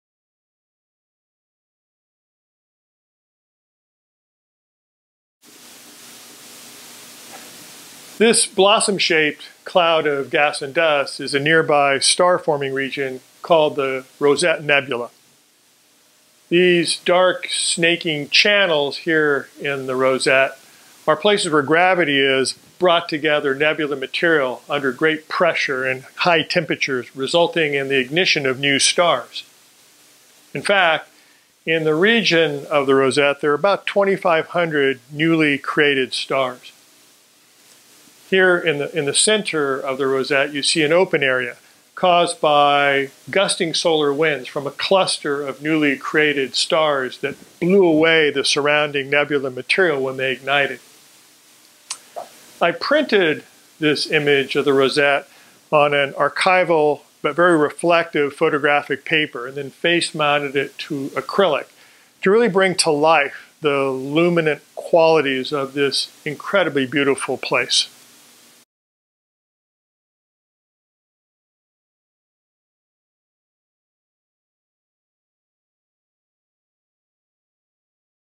Rosette: One-Minute Art Tour
Gary Lopez gives a quick tour of his fine art astrophotograph, Rosette.